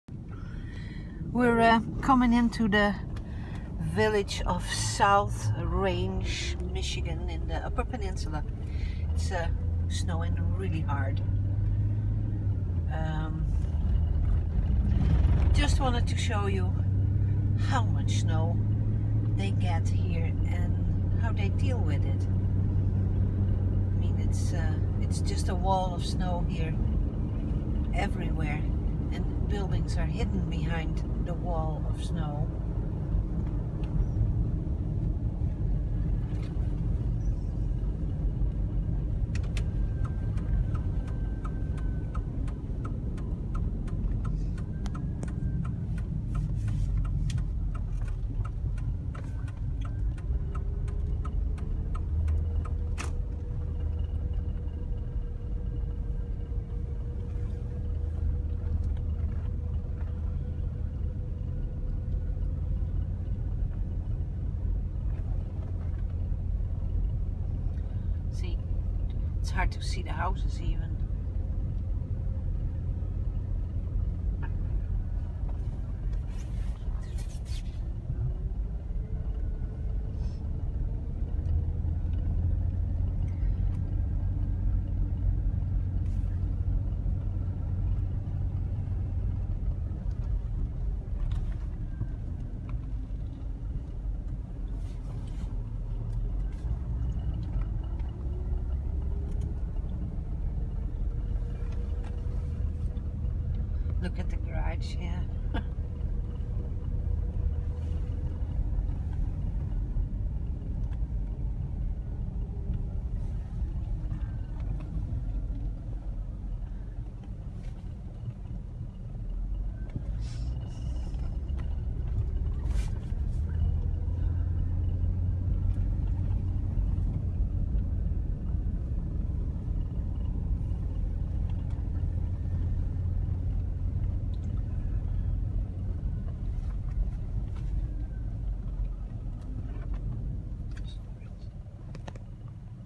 South Range, Michigan Driving in the Snow
South Range is in Michigan's Upper Peninsula, just south of Houghton.